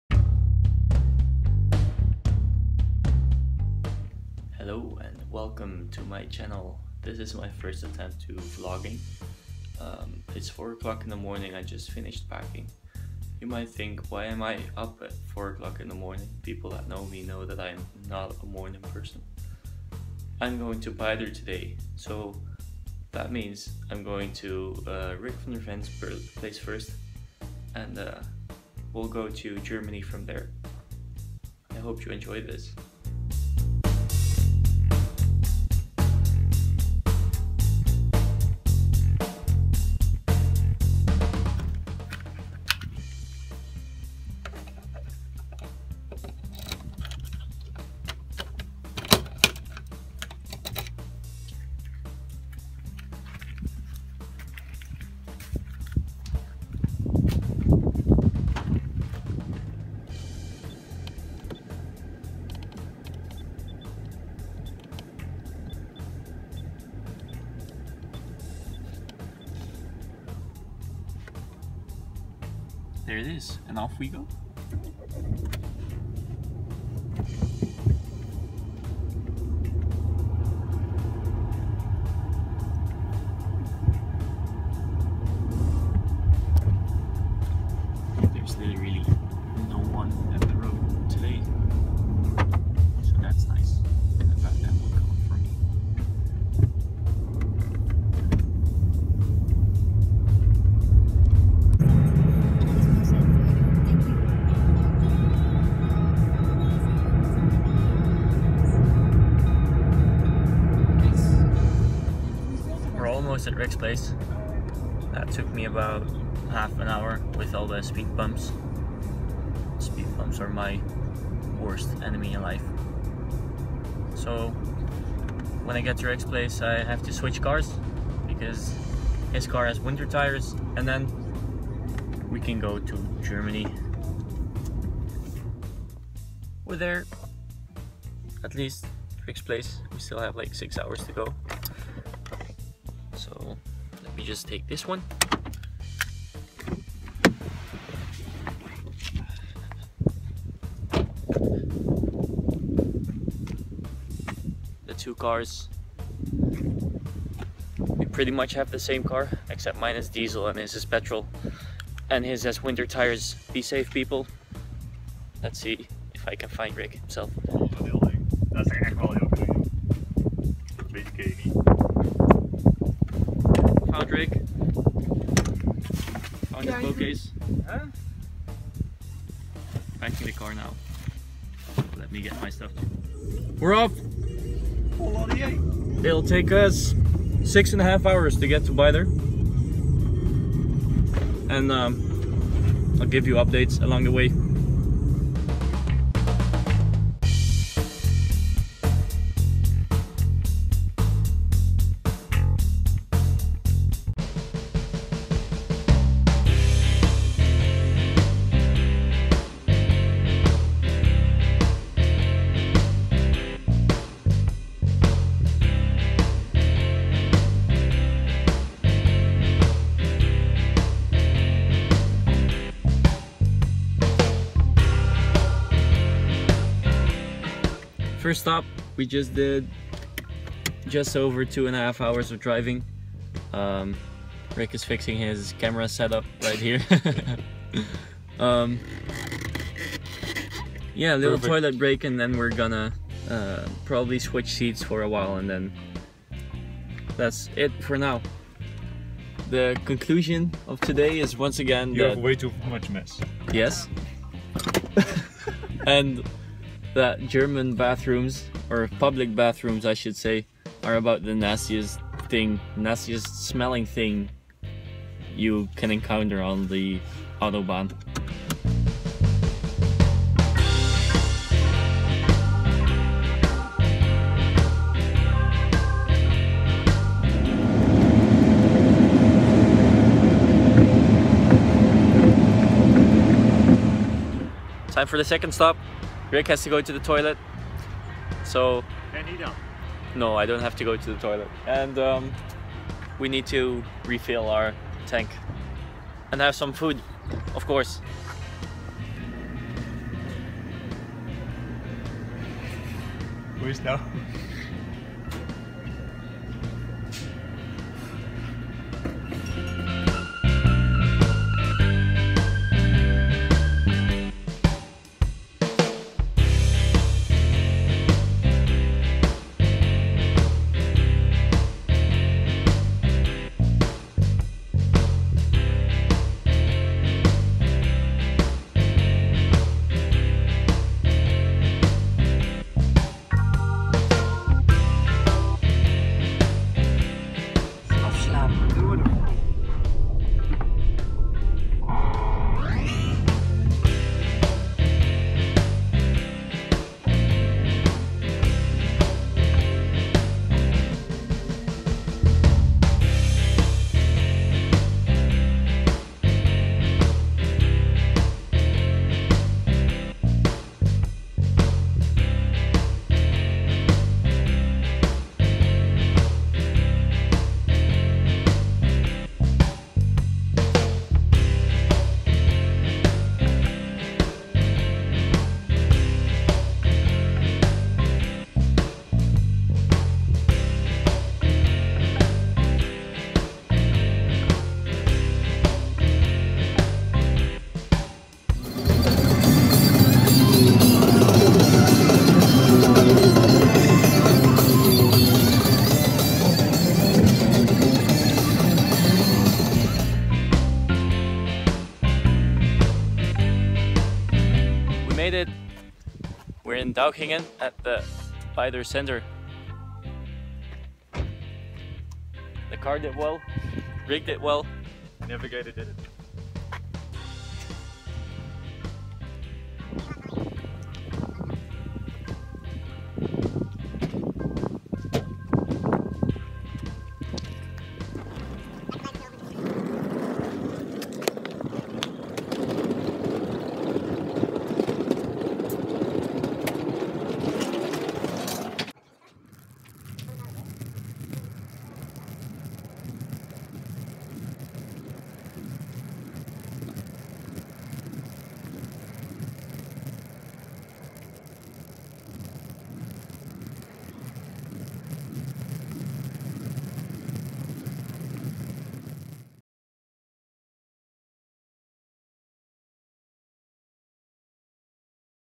Trip to Beiter! (First vlog!)
Made a video about our trip to the Beiter centre, together with Rick van der Ven. First attempt to vlogging :) Let me know what you think by sending an email to or visit my Facebook, twitter or instagram!